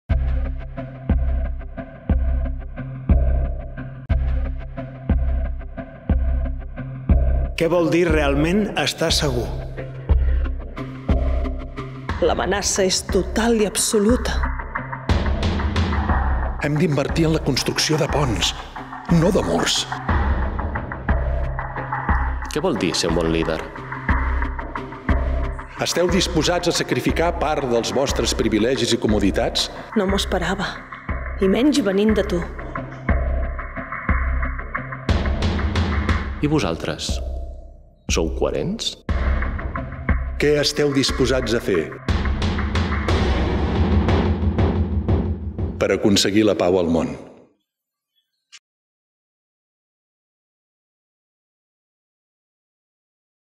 ELECCIONS - Un espectacle teatral interactiu per a reflexionar i debatre sobre la seguretat
Quina seguretat volem, la que aposta pel militarisme i l’enfrontament, o la que posa les persones al centre i treballa per garantir els seus drets fonamentals?

Aquesta és la reflexió central de l’obra ‘ELECCIONS’, que hem preparat conjuntament a FundiPau amb la companyia de teatre Frec a Frec, i que estrenem aquest mes de setembre 2024 a Lloret de Mar i Barcelona.

La vols veure al teu municipi o equipament?

Amb el suport de l'ICIP i el Departament de Cultura de la Generalitat de Catalunya.